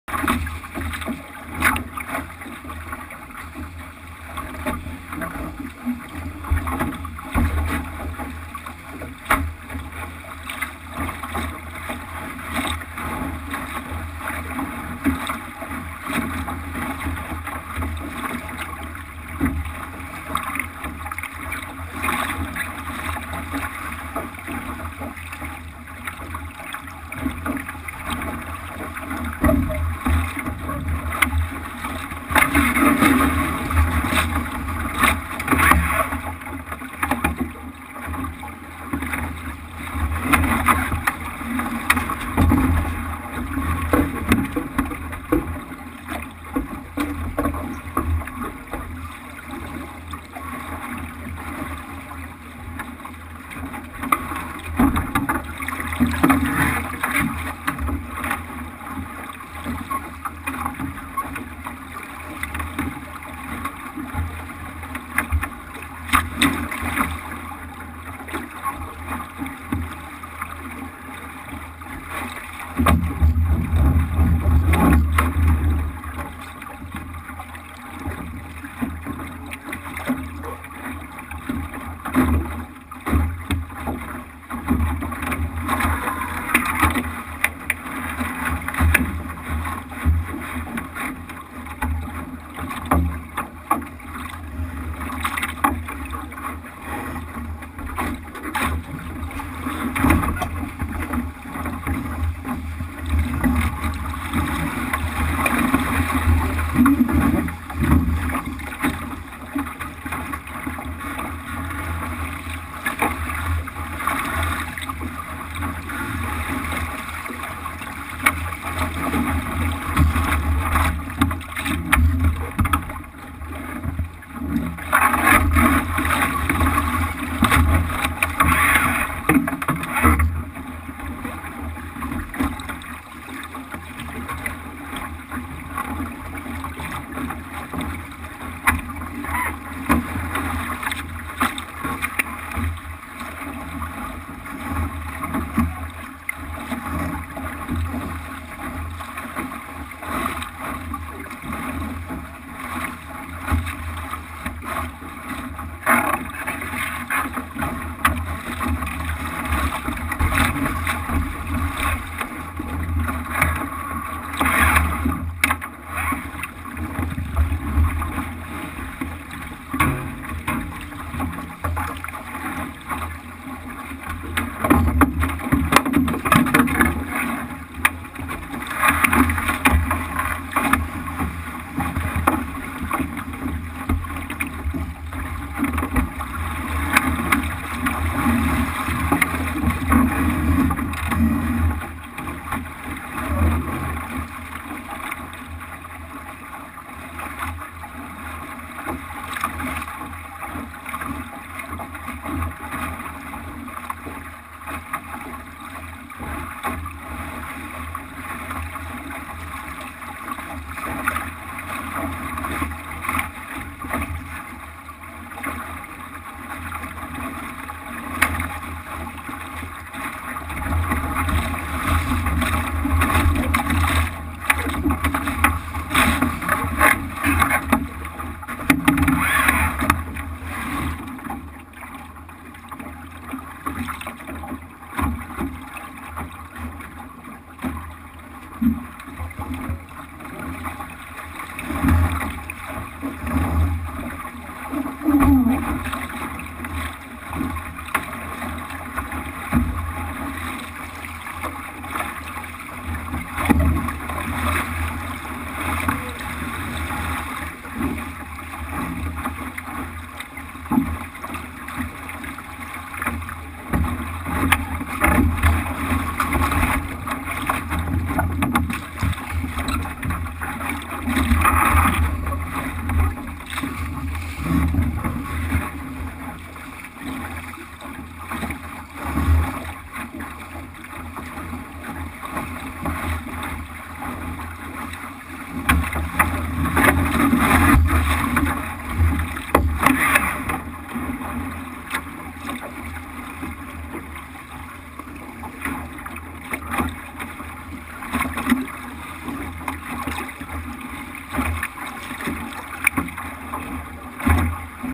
Pirat - Türkiye Pirat Ligi 3.Etap 2015
Ülkemizin çift kişilik en kalabalık sınıfı olan Pirat sınıfında yarışan ekibimizin, 2. olarak tamamladıkları Türkiye Pirat Ligi yarışındaki heyecanlı anları.

Ekip:
İrfan Papila
Burak Cora